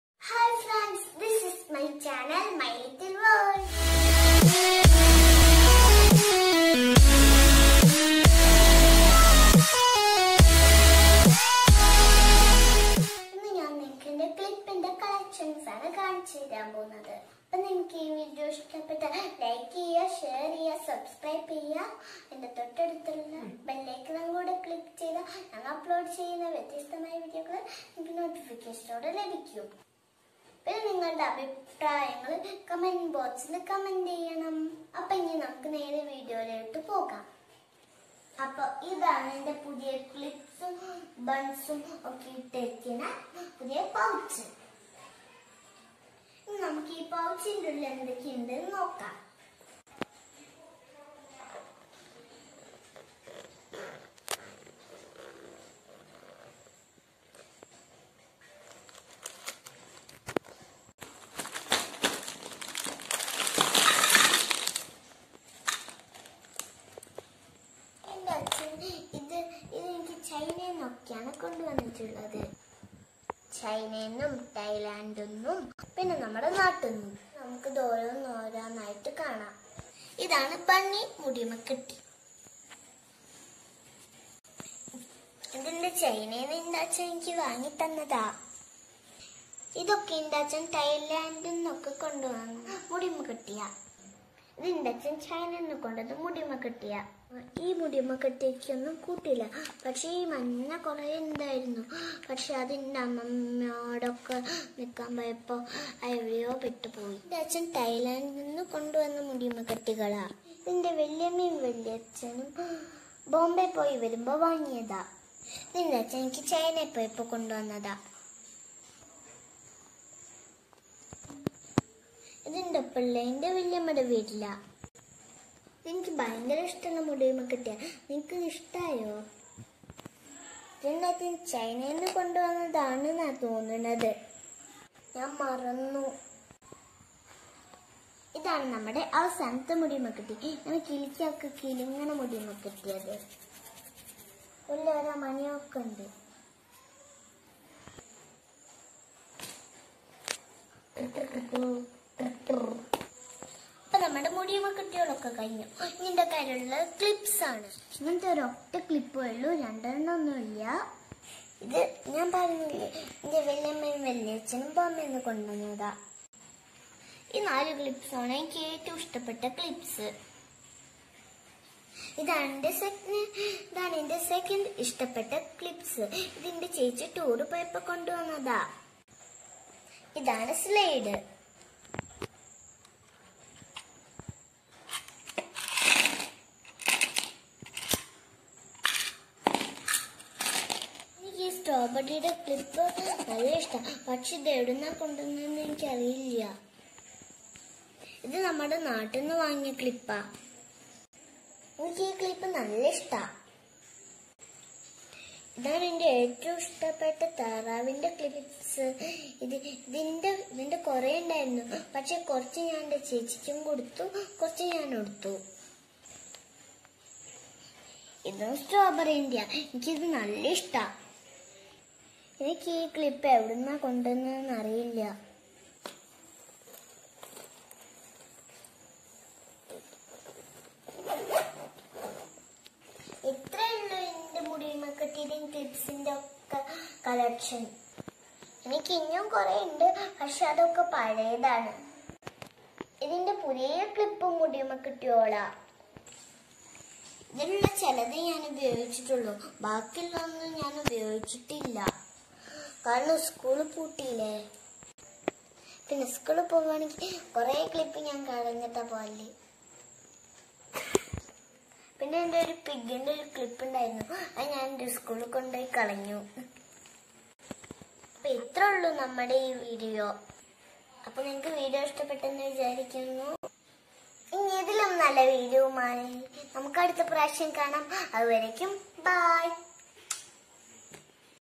My clip and bun collection!!/my little world by Avanthika#my collection
Hi friends hope you enjoyed this video

These are my collections of new clips and buns

Bye!!